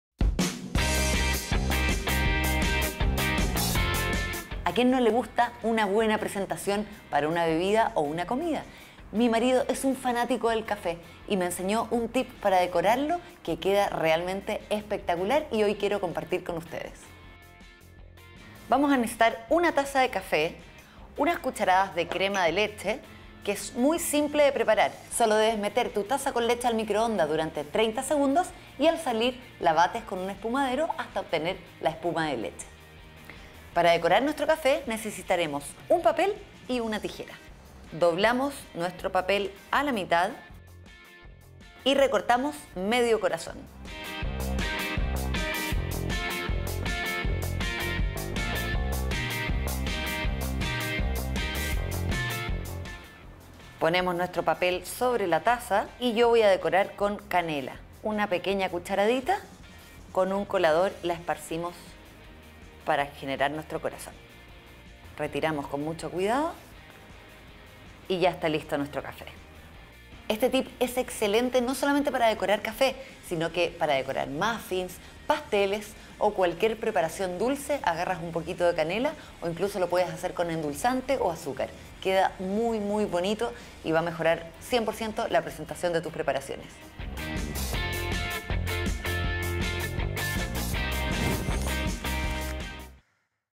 DECORA TU CAFÉ / MANOS A LA OBRA
Un tip para darle el toque especial a preparaciones como el café